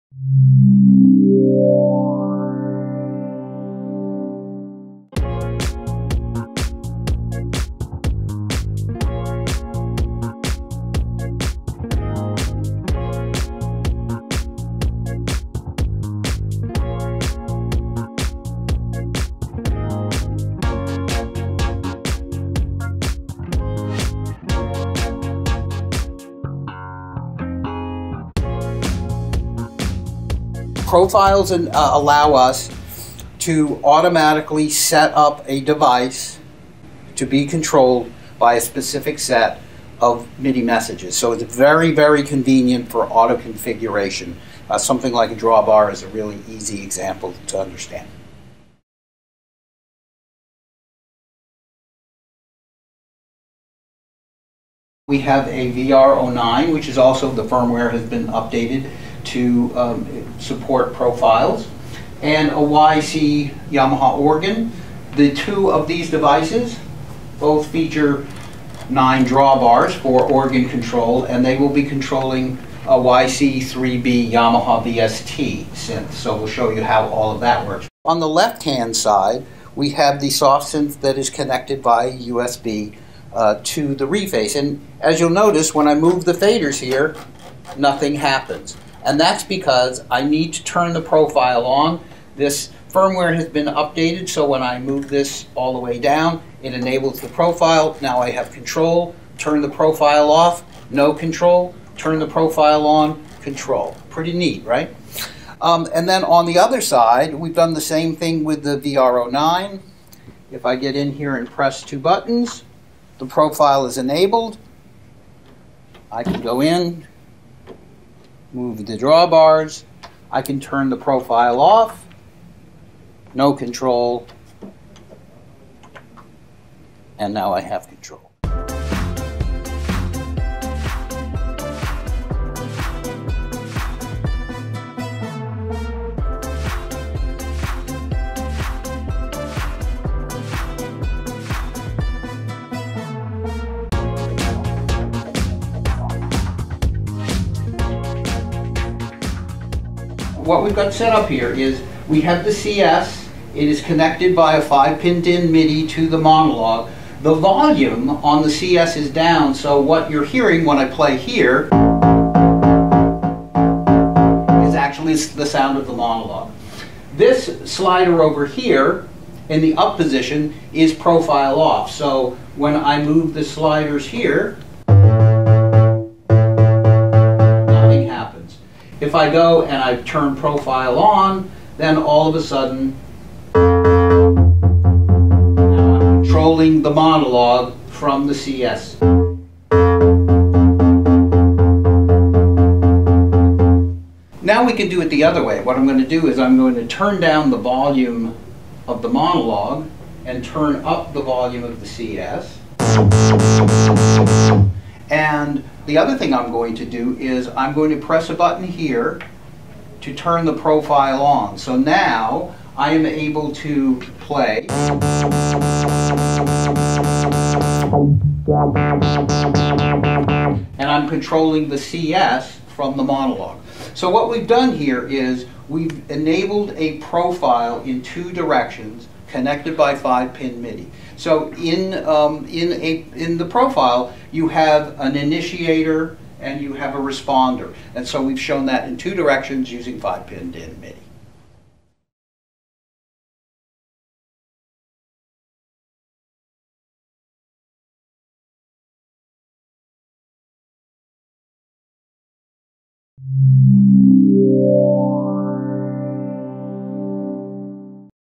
About MIDI 2 0 Profiles
Profile Configuration

MIDI gear can now have Profiles that can dynamically configure a device for a particular use case. If a control surface queries a device with a "mixer" Profile, then the controls will map to faders, panpots, and other mixer parameters. But with a "drawbar organ" Profile, that same control surface can map its controls automatically to virtual drawbars and other keyboard parameters—or map to dimmers if the profile is a lighting controller. This saves setup time, improves workflow, and eliminates tedious manual programming.